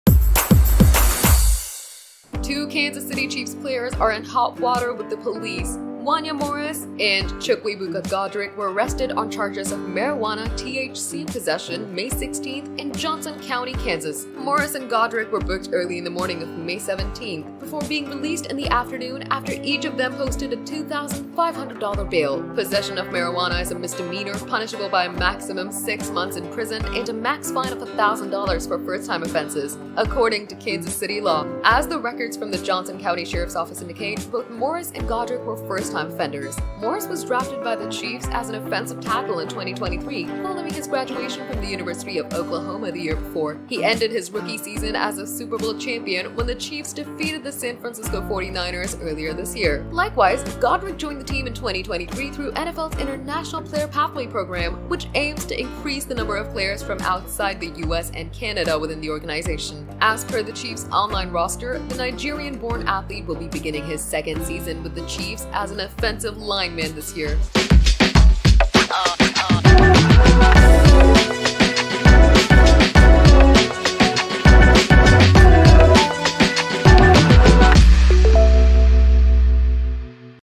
Entertainment | Kansas City Chiefs Players SHOCKING Arrest | Gossip Herald
Entertainment | Kansas City Chiefs' Wanya Morris and Chukwuebuka Godrick Arrested for Marijuana Possession.